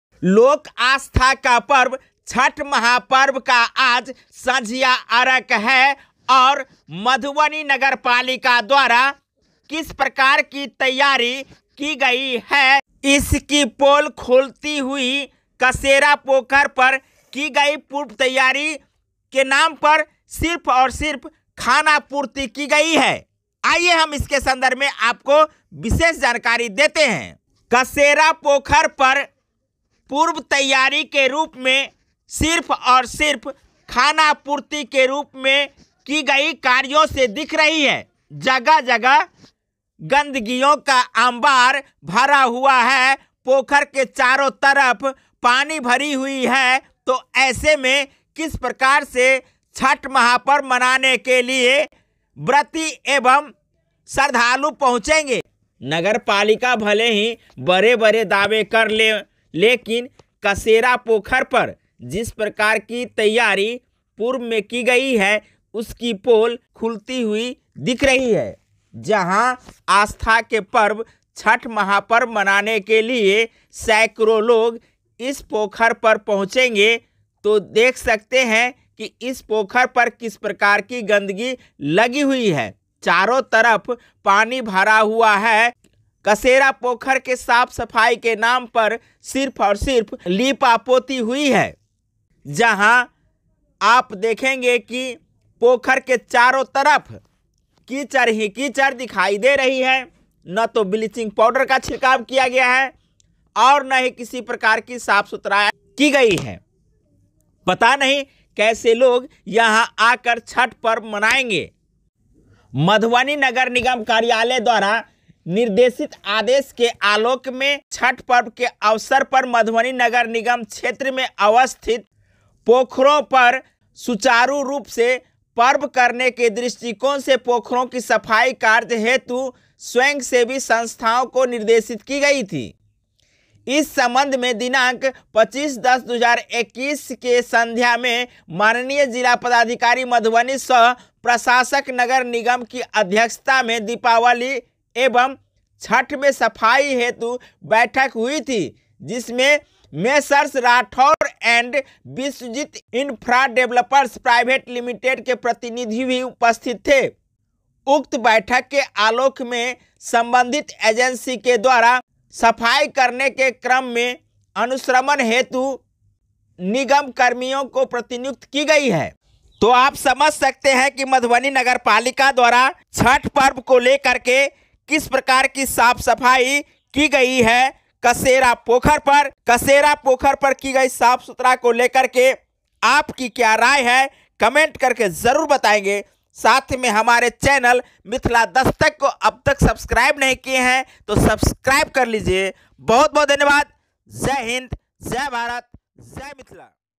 छठ घाट की साफ-सफाई के नाम पर कसेरा पोखर की गई खानापूर्ति गंदगी का अंबार
छठ व्रत की खरना हेतु बनोर गांव में कलश यात्रा में सैकड़ों व्रति ली भाग।